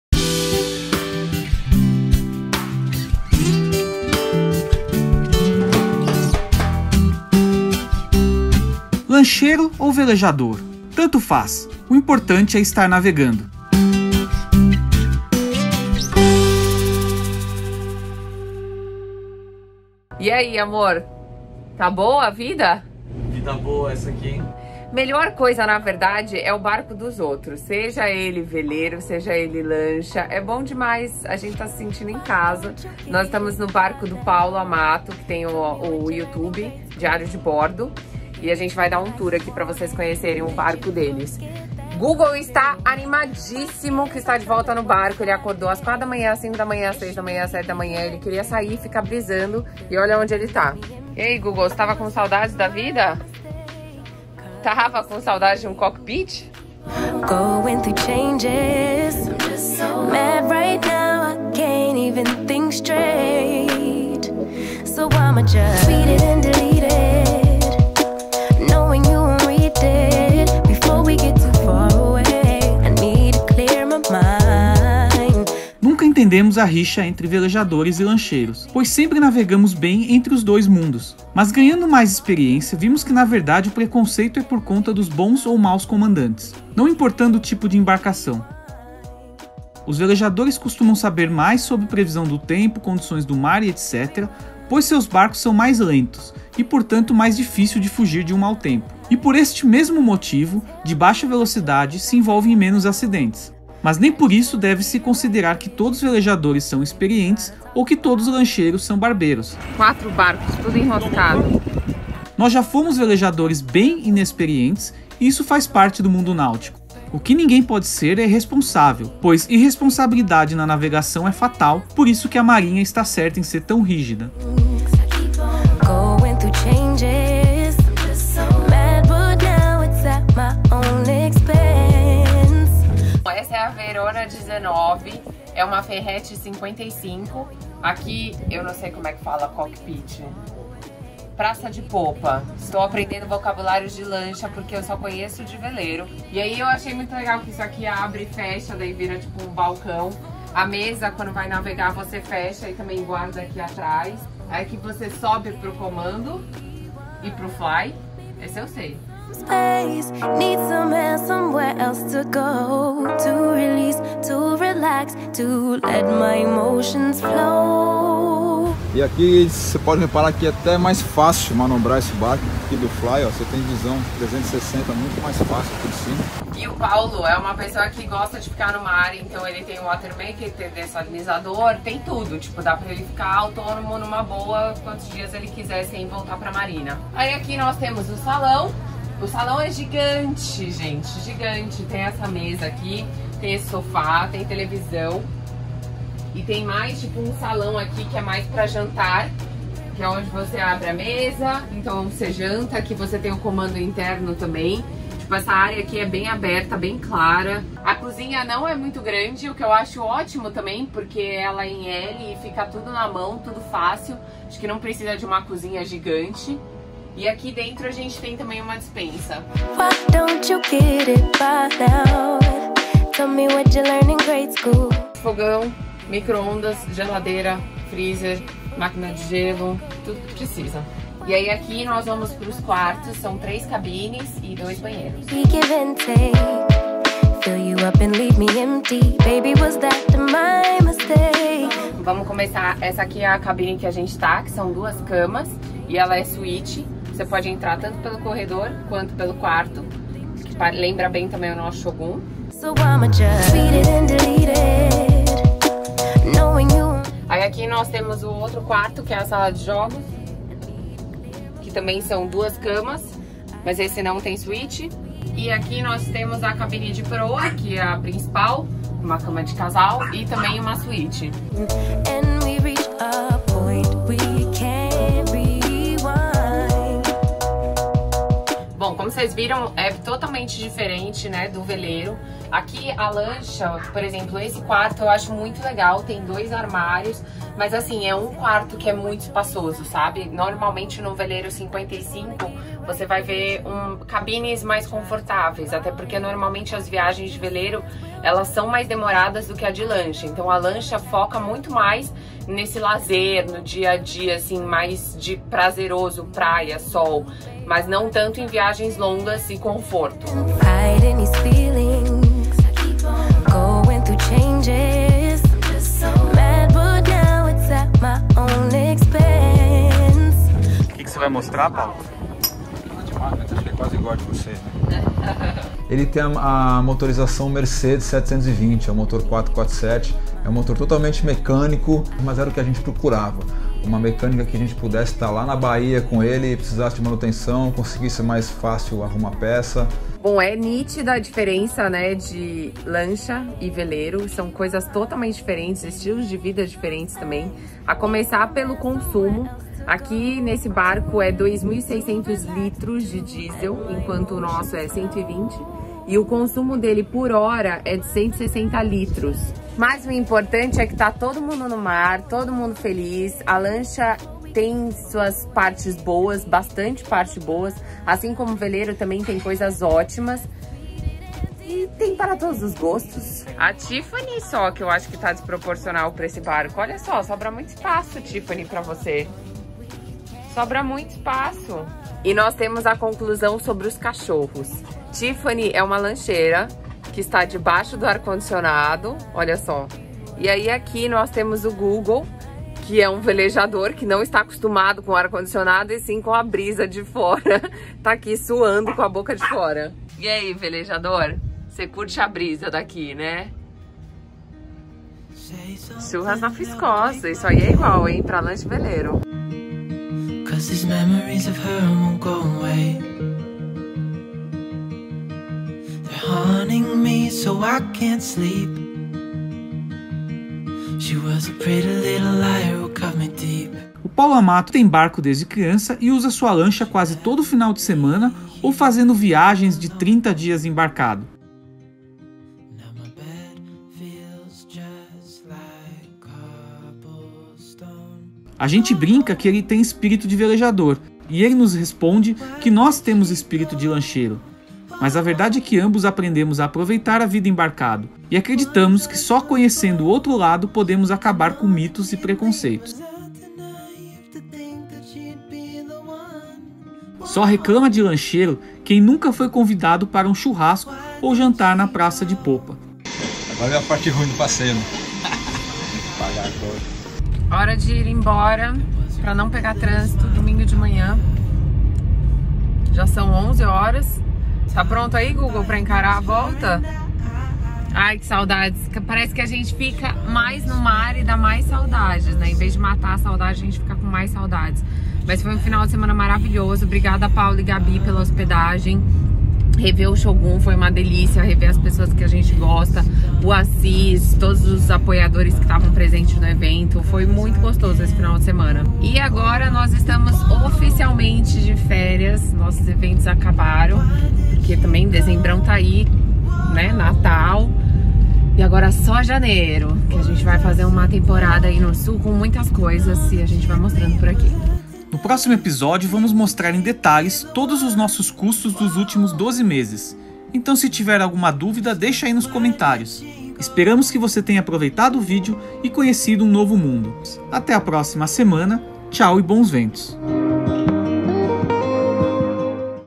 138 | Vida de Lancheiro - Ferretti 55 - Sailing Around the World
VELEJANDO SAILINGAW VELEIRO Vida de lancheiro ! Conheça a Ferretti 55 do Paulo Amato - Diário de Bordo

Essa coisa de veleiro x lancha tem que acabar, nossa intenção sempre foi acrescentar o mundo náutico em geral e com esse video mostramos um pouco mais sobre a vida de lancheiro !! Que também não é nada mal !!!

O Paulo Amato do canal Diario de Bordo tem uma Ferretti 55 a qual passamos o final de semana e foi maravilhoso!!
Aproveitamos para rever amigos que nos ajudaram durante essa jornada !!!

Espero que goste, bons ventos

Abraços e Bons Ventos!
Beto Toledo / Thais Canado / Google Golden